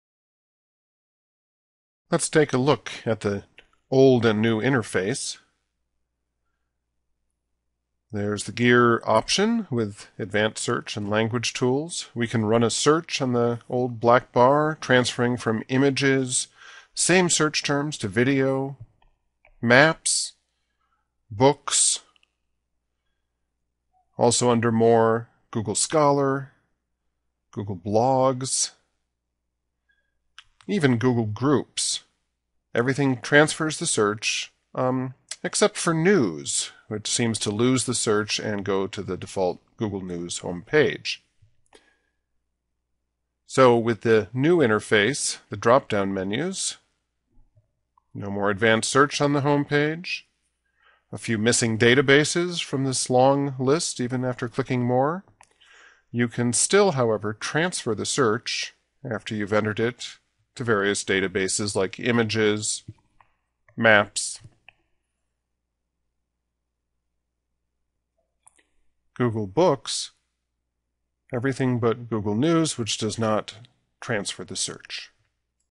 New Google Interface vs. Old
Shows search switching with the old black and compares to the new in Dec. 2011 Google interface.